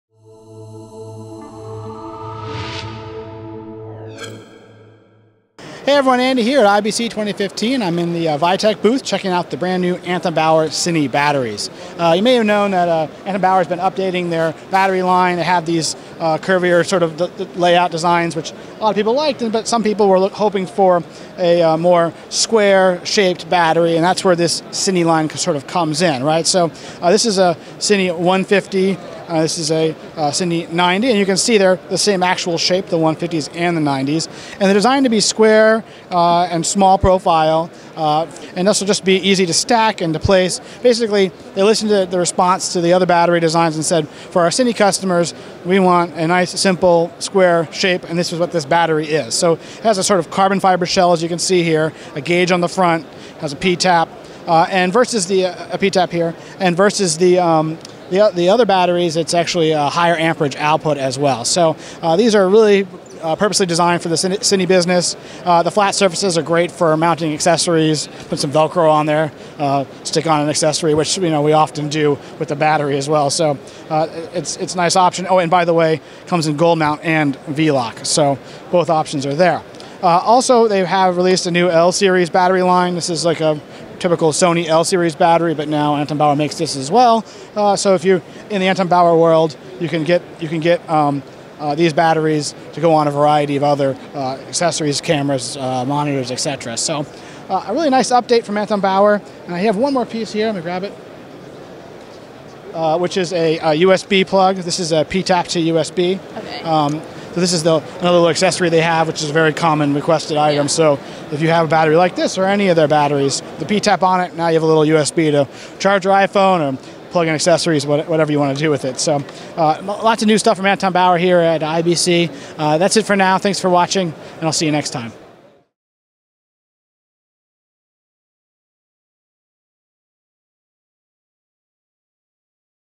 IBC 2015: Anton Bauer CINE Batteries
Anton Bauer has introduced their new line of CINE batteries designed specifically for professional cinematographers. The CINE series delivers 12 amps of continuous power and features Fuse Link technology, meaning that if something were to go wrong with one of the cells, the surrounding cells would be protected from damage. Anton Bauer definitely listened to a lot of feedback from their users in designing these new batteries, so watch my video above to learn more.